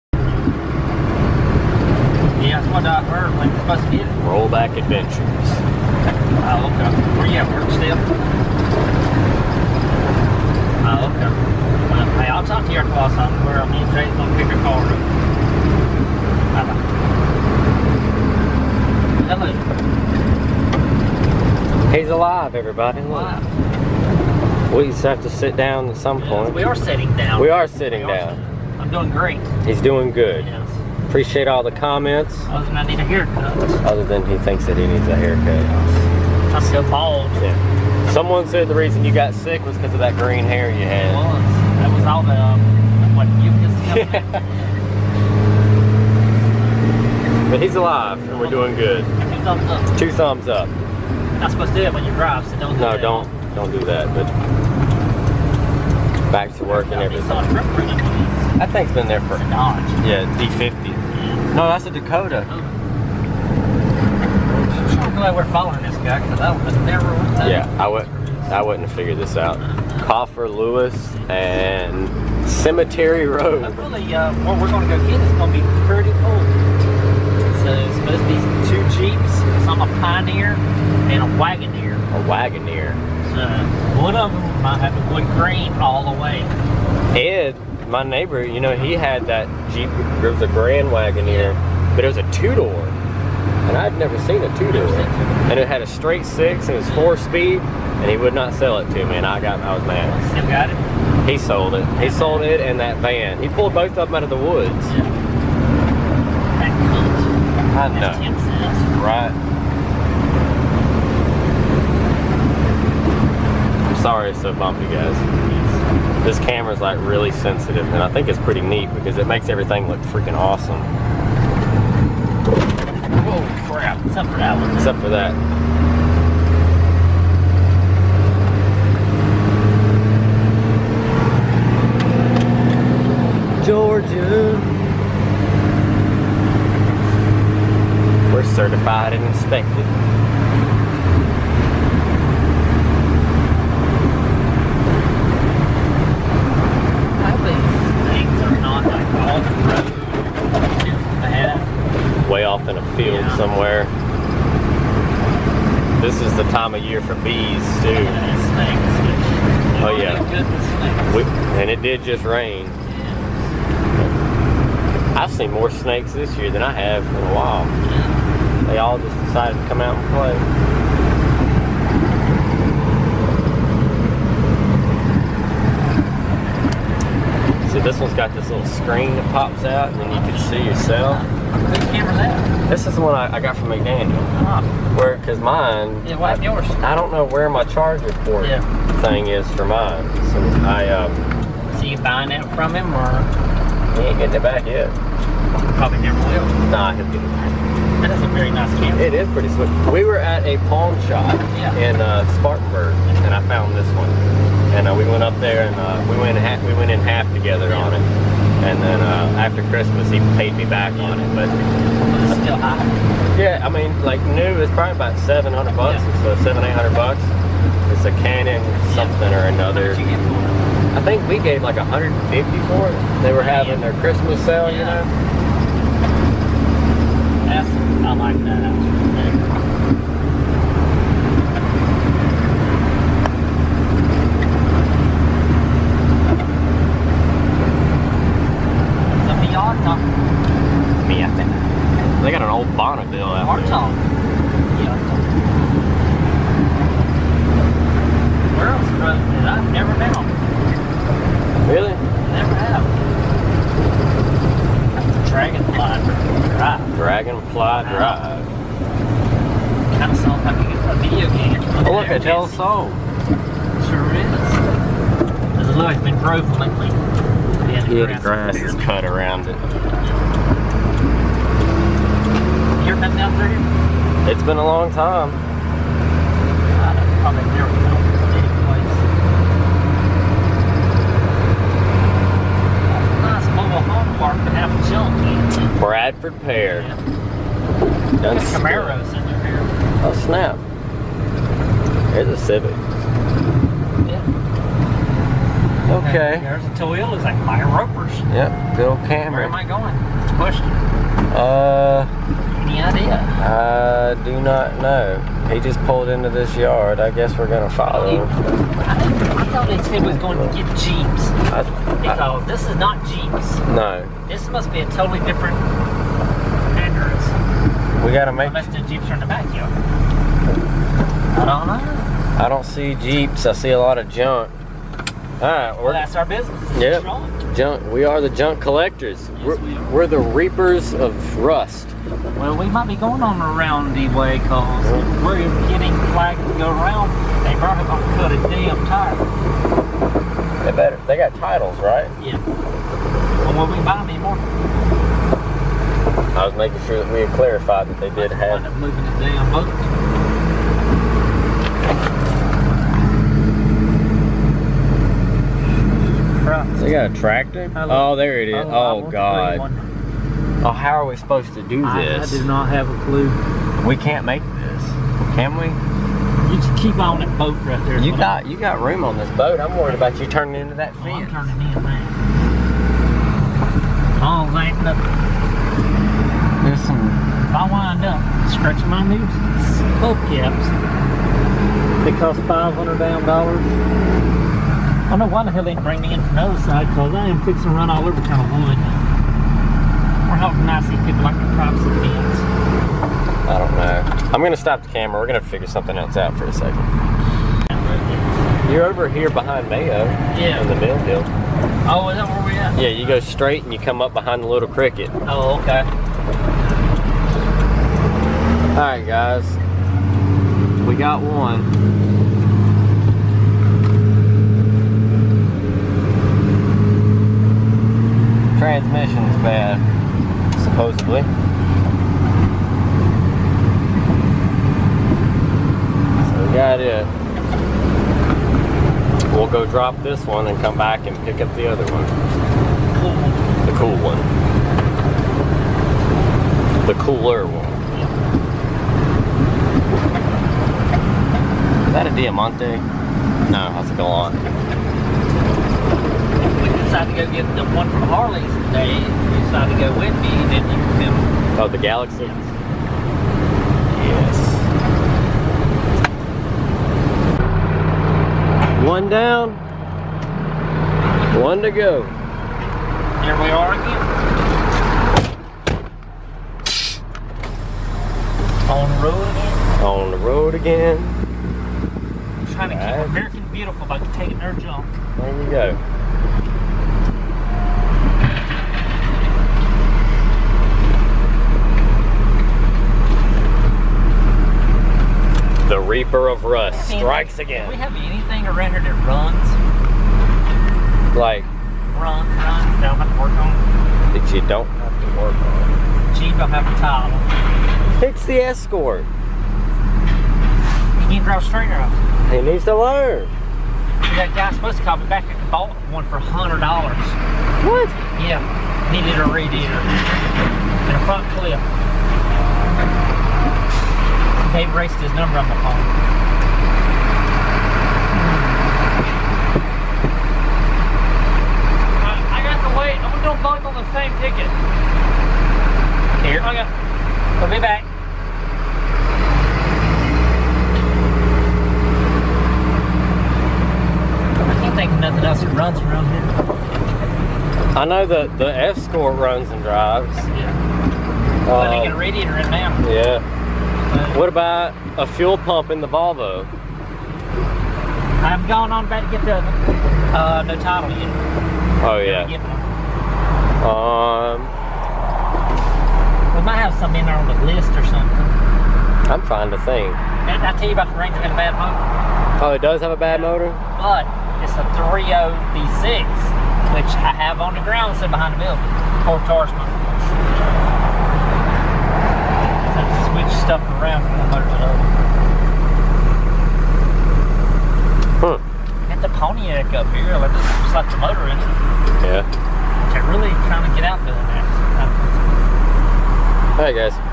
Hes Doing Good! + Towing AMC's Vlog ?
Take a ride with us today to see what were going to be picking up for the recycling center !